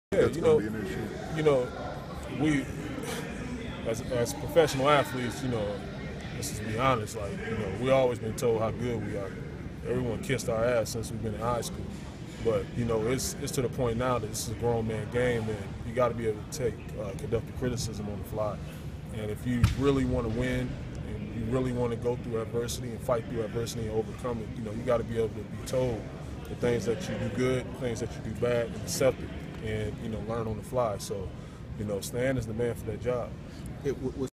Pistons Caron Butler on players handling Stan Van Gundy's criticism
By: Dave Pemberton
Published on: September 30, 2014